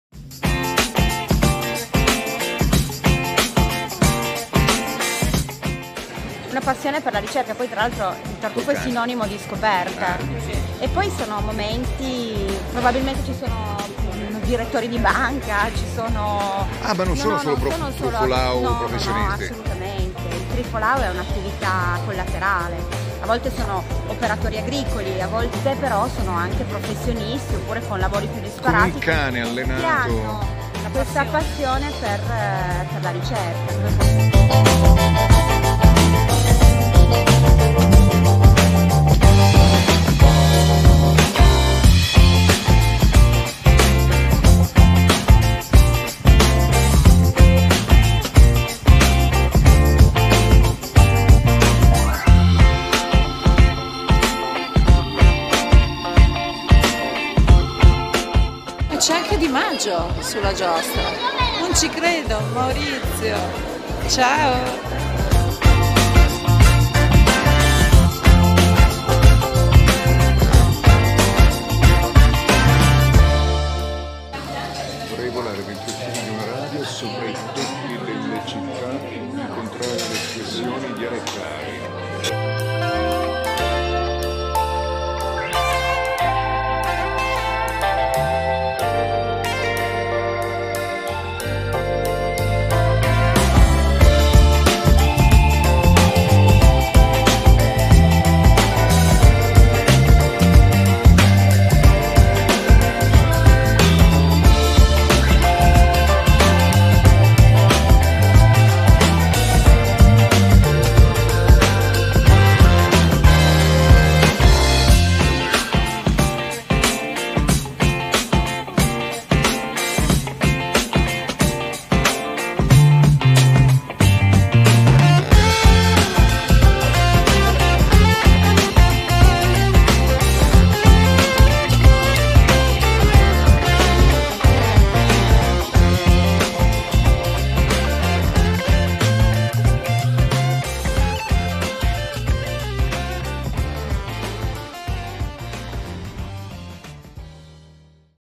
Fiera del Tartufo day 2
Il mio secondo giorno alla Fiera Internazionale del Tartufo Bianco di Alba